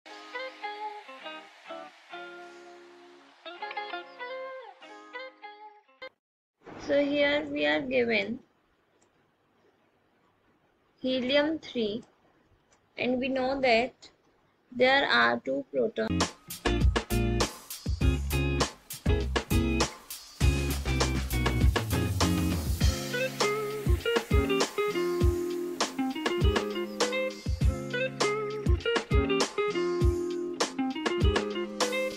Our Sun is a fairly small star that has barely enough mass to fuse hydrogen into helium. Along the …
Our Sun is a fairly small star that has barely enough mass to fuse hydrogen into helium. Along the way, an isotope of helium, helium-3, is formed. 1st attempt Calculate the binding energy per nucleon of helium-3 on the basis of the following masses: Particle Mass (u) ZHe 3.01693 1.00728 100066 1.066 * 10^-3 1.998 * 10^8 Binding energy # Aaus Ica